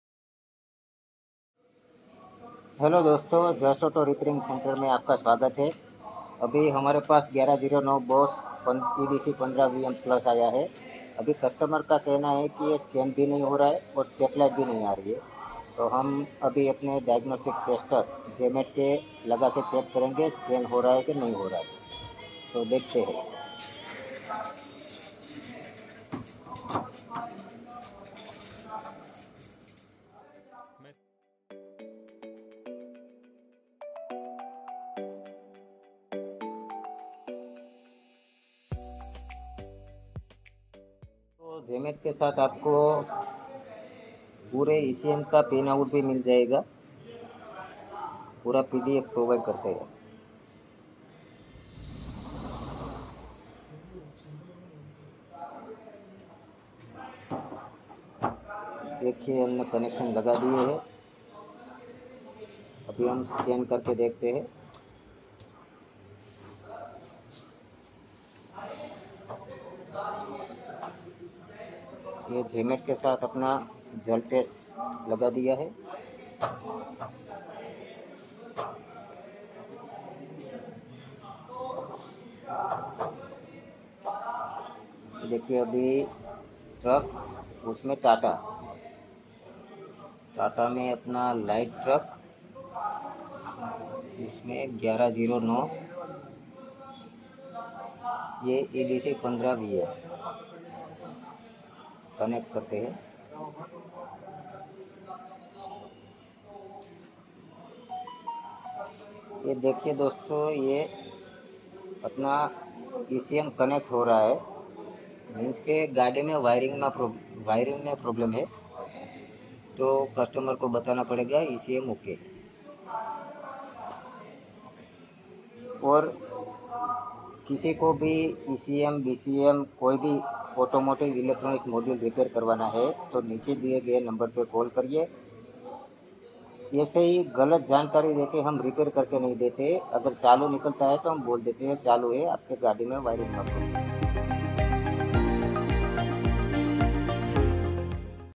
ट्रक, बस, कार, प्रीमियम बाइक्स के ECM / BCM / इलक्ट्रोनिक मॉड्यूल की रिपेयरिंग at JustAuto Solutions
ECM / BCM / FRM या कोई भी ऑटोमोबाइल इलेक्ट्रॉनिक मॉड्यूल रिपेयर करवाना हो तो JustAuto के कमर्शियल सेक्शन में भेज देवें। ट्रक, बस, कार, प्रीमियम बाइक्स के ECM / BCM / इलक्ट्रोनिक मॉड्यूल की रिपेयरिंग बहुत ही वाजिब दाम में करके आपको वापस कूरियर कर दिया जायेगा।